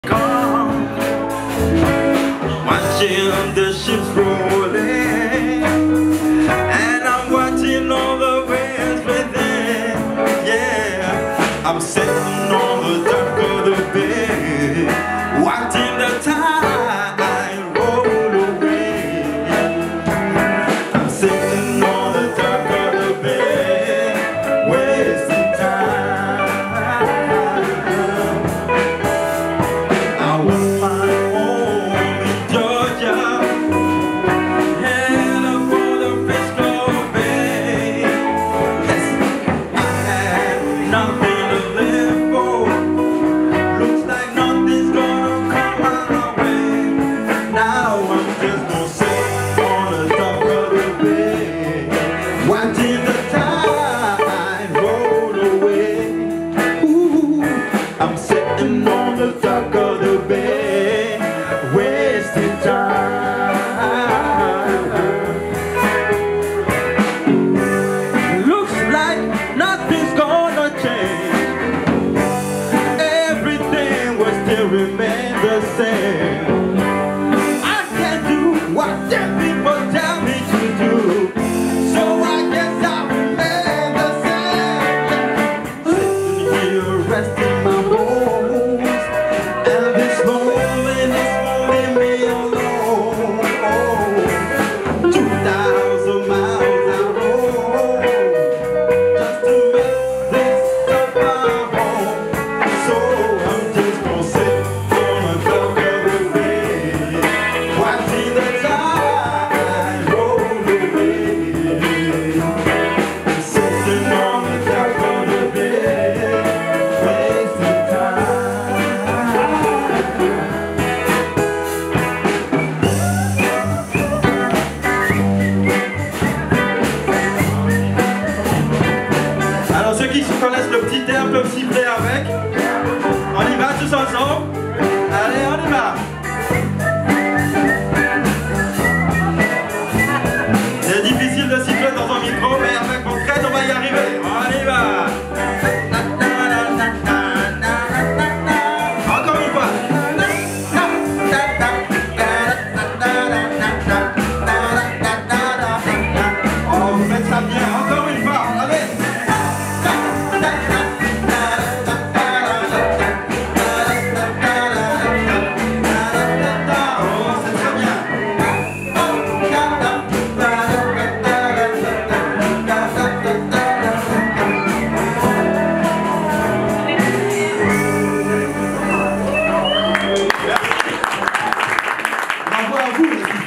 The Soul Must Go On, The dock, Brussels, 26.10.2019
The Soul Must Go On is a funk/soul band based in Brussels. Eki (lead singer), Eduardo (drums), Benoît (lead guitar), Pierre-Marie (keyboards), Olivier (bass), Gaël (trumpet) and Paras (rhythm guitar) love to play songs by Marvin Gaye, Al Green, Stevie Wonder and other soul musicians. Video: Anna. Here they perform a famous Otis Redding song in the well-known Falstaff restaurant..